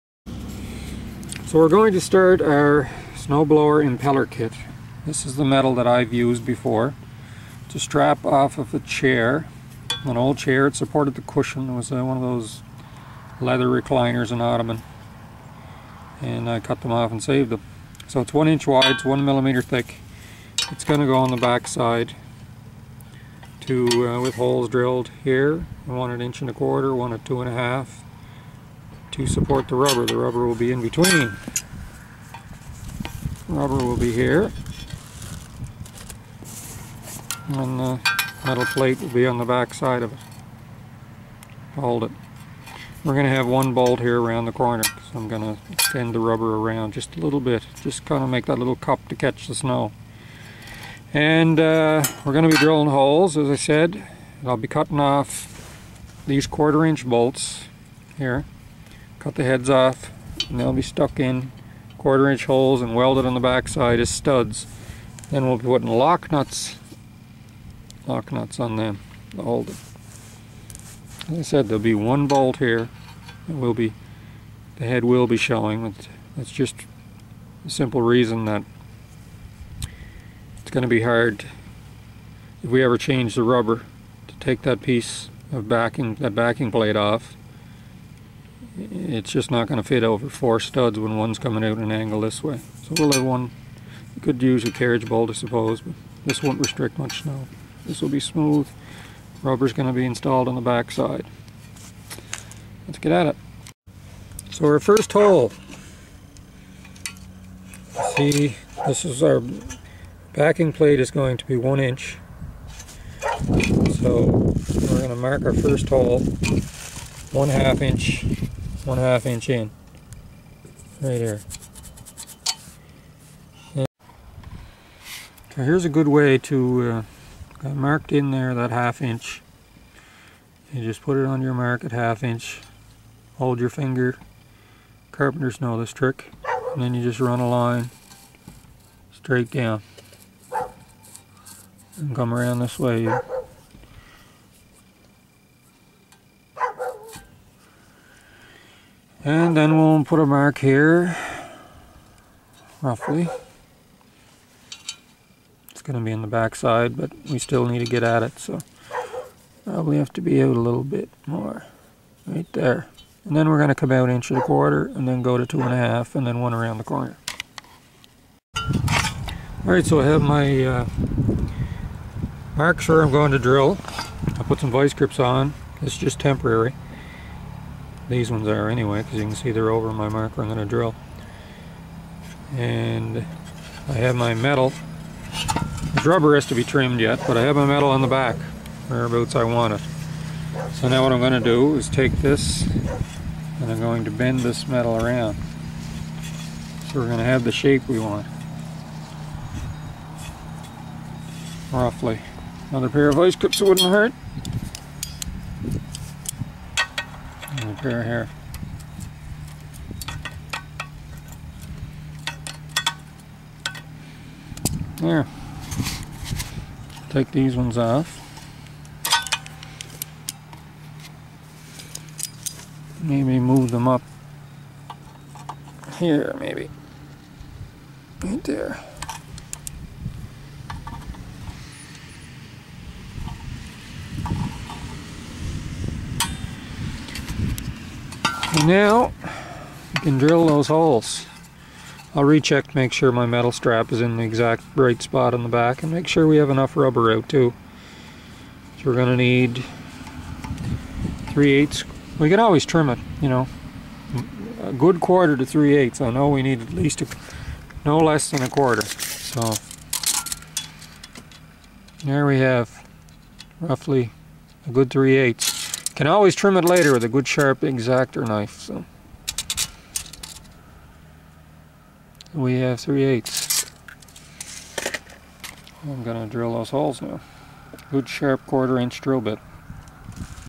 Snowblower Impeller Modification Part 2
Part 2 of my series installing a rubber snowblower impeller modification.